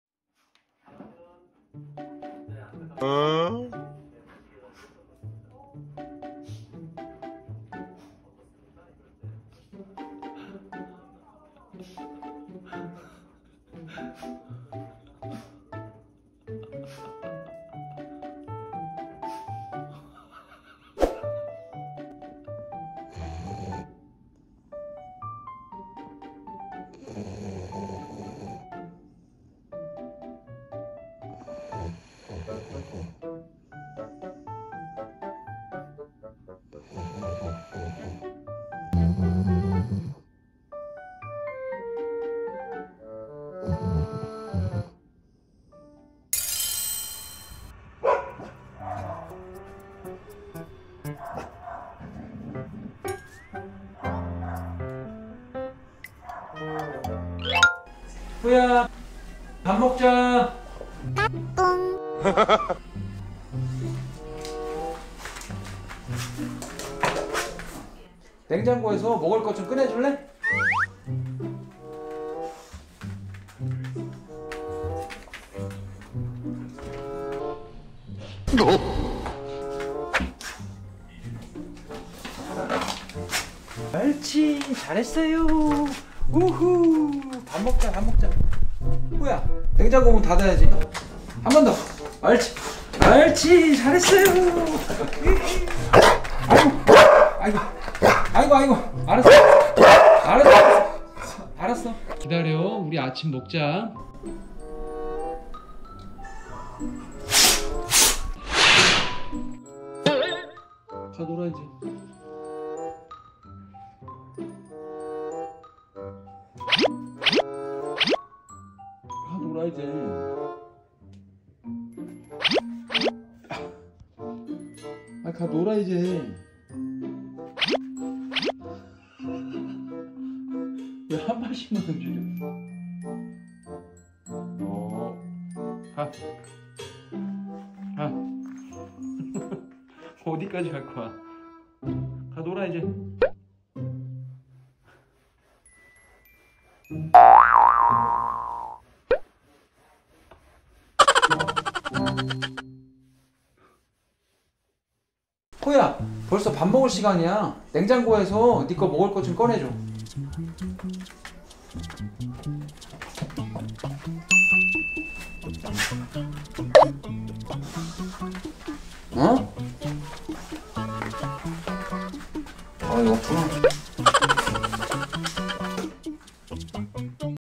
냉장고에서 밥 꺼내오라고 했는데 문열어보니 아무것도 없을때 리트리버 반응
마지막 장면이 촬영 첫 장면이었는데
냉장고 문을 열고 호야 표정이 이상해서
확인해 보니 아무것도 없어서
저도 당황했습니다.
호야는 더 당황했겠죠? ^^
그 와중에 얌전히 뒤에서 지켜보는
우리 하루는 너무 예뻐~